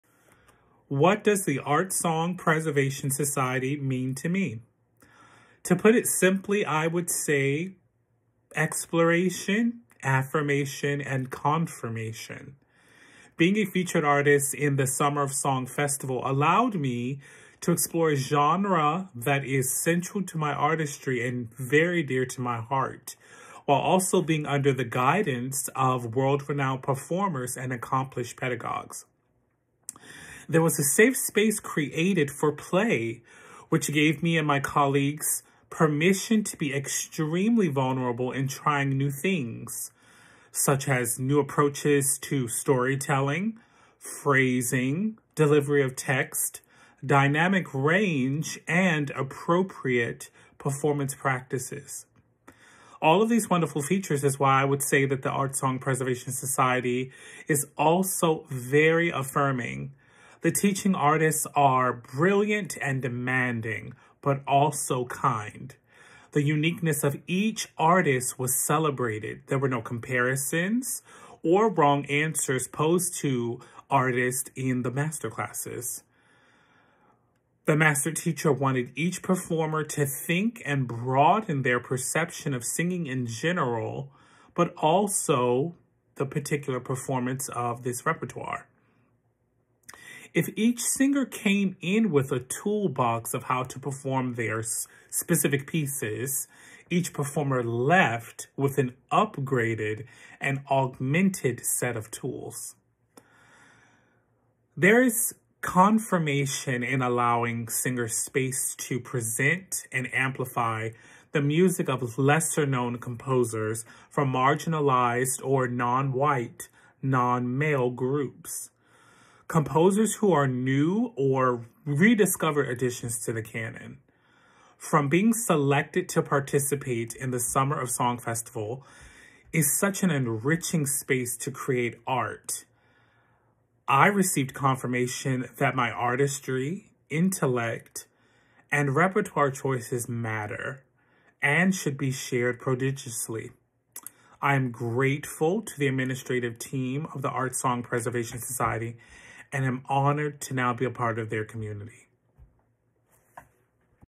Thomas Lloyd - Summer of Song 2024 Testimonial
We are thrilled to share a series of heartfelt testimonials from the talented individuals who participated in our Summer Art Song Festival. Their feedback highlights the incredible experiences, growth, and inspiration that the festival brought to artists and audiences alike.

Each testimonial offers a unique perspective on the workshops, masterclasses, and performances that made this festival an unforgettable journey. From the cultural transformation workshops to the diverse art song recitals, participants have expressed how the festival enriched their understanding and appreciation of this beautiful genre.

Stay tuned as we post these testimonials, and immerse yourself in the voices of those who made the Summer Art Song Festival presented by The Art Song Preservation Society and the Duncan Williams Vocal Program and hosted by Manhattan School of Music a resounding success!